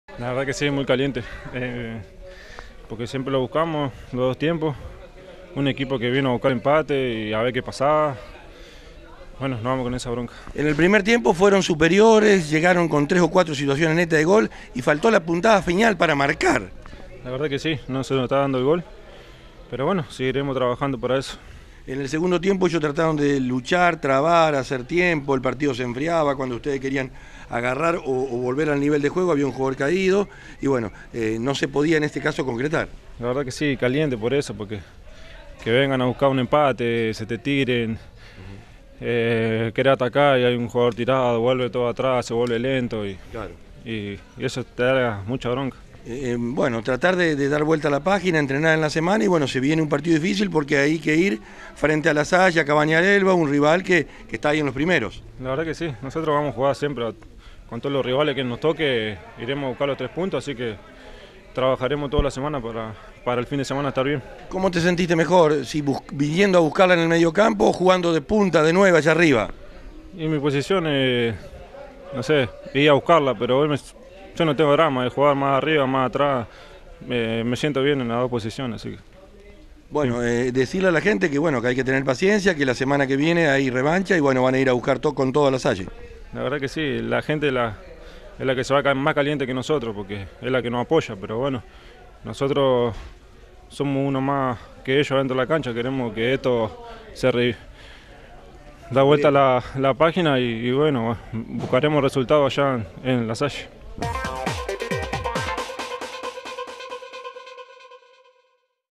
JONATAN TARQUINI – Volante
Sanjustino empató sin goles ante Sportivo Guadalupe por la 5ta. Fecha del Torneo Clausura de la Liga Santafesina de Fútbol